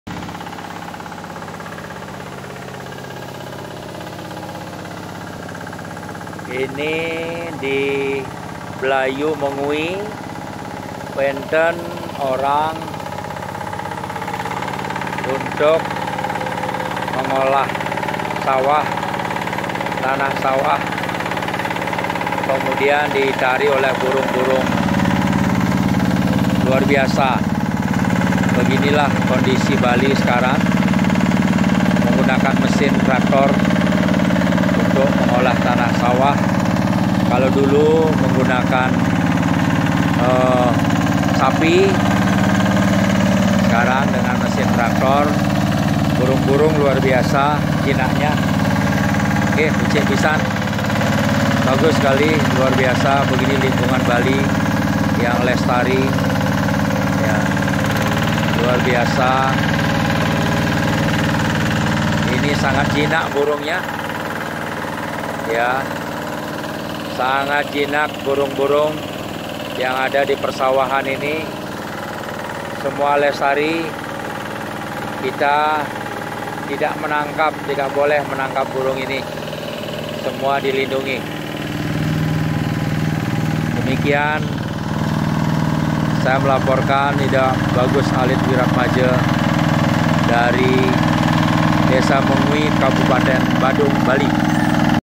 Burung bangau putih akrab dengan petani.
Burung bangau putih atau di Bali disebut Burung Kokokan akrab dengan petani yang sedang mentraktor sawah di wilayah Mengwi, kabupaten Badung. Burung ini  dilestarikan, terutama di tempat bersarangnya di desa Petulu, kabupaten Gianyar telah menjadi obyek wisata.